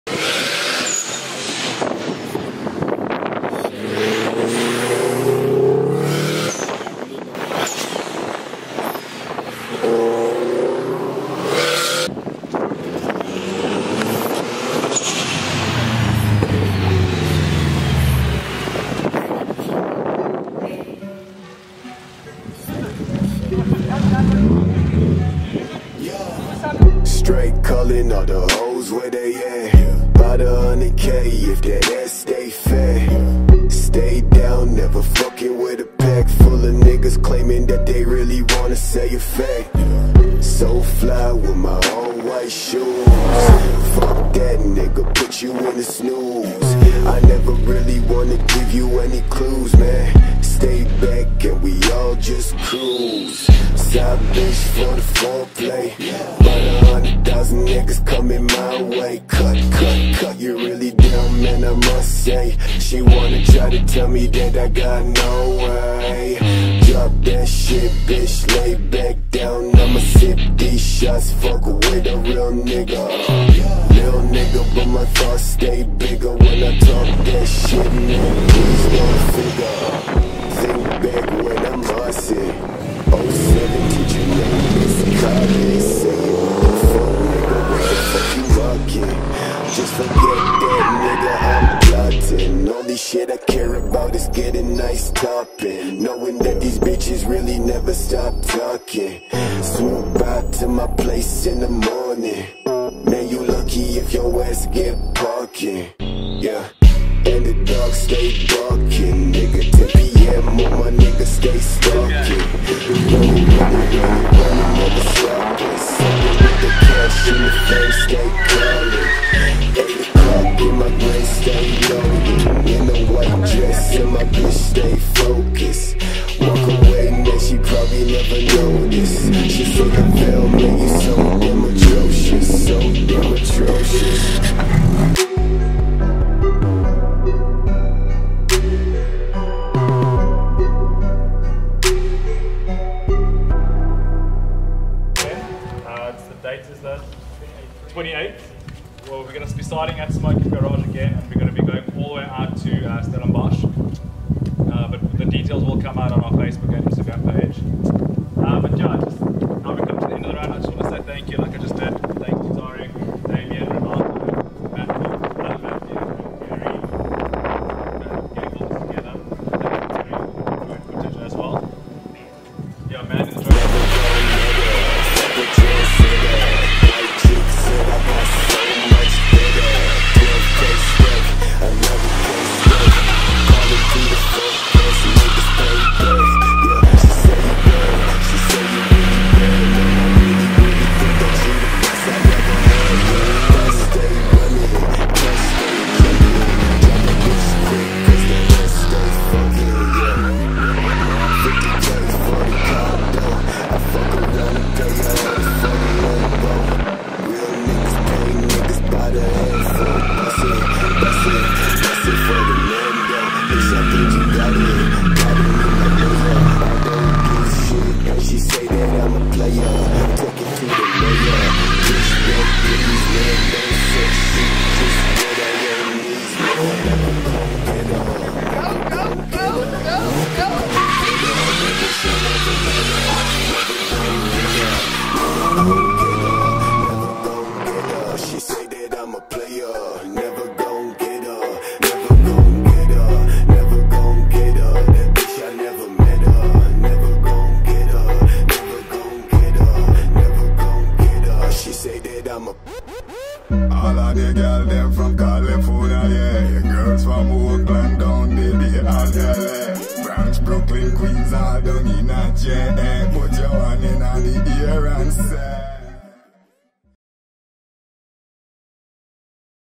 Capestance & Smoke'm Garage Breakfast Run
Capestance and Smoke'm Garage hosted a breakfast run from Smoke'm Brewhouse to ATM Chiptuning traveling via westcoast.